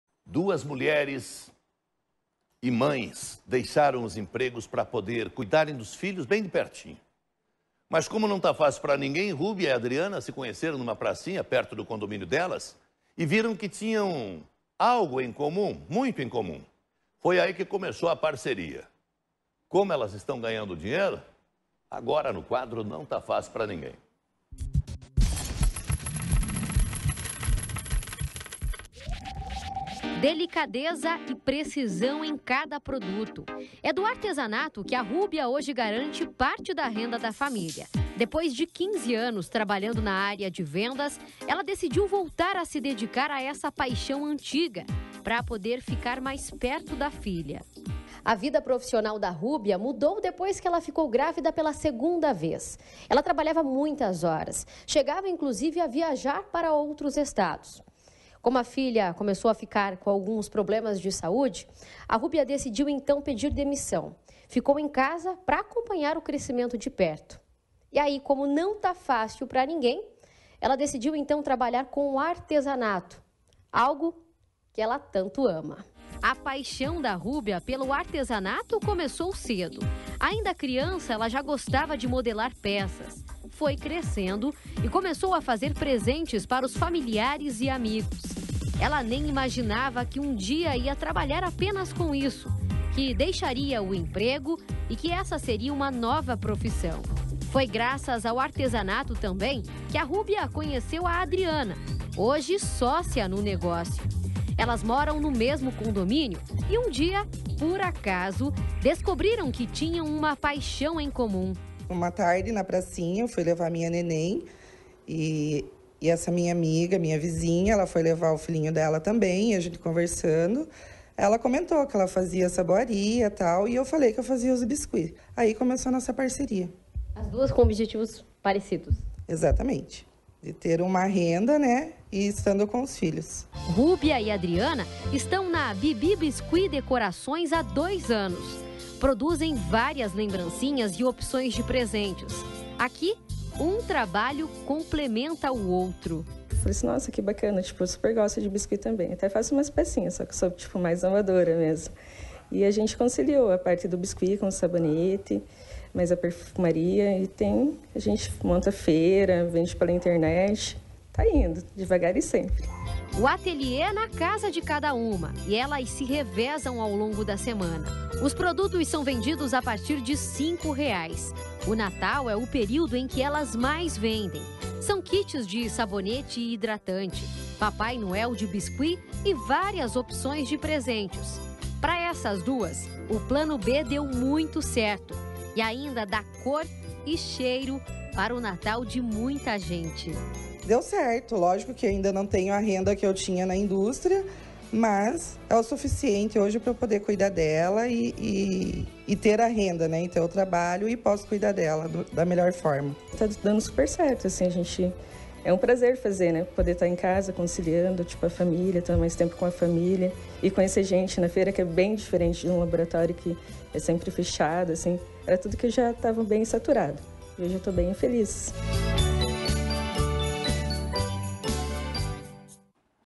Não tá fácil pra ninguém: mães ganham dinheiro com artesanato e cuidam dos filhos
Duas mães deixaram os empregos para cuidar dos filhos de perto. Mas como não tá fácil pra ninguém, Rúbia e Adriana se uniram e começaram a vender biscuit e sabonetes juntas. Elas se conheceram em uma pracinha perto do condomínio onde elas moram e viram que tinham muito em comum. Veja!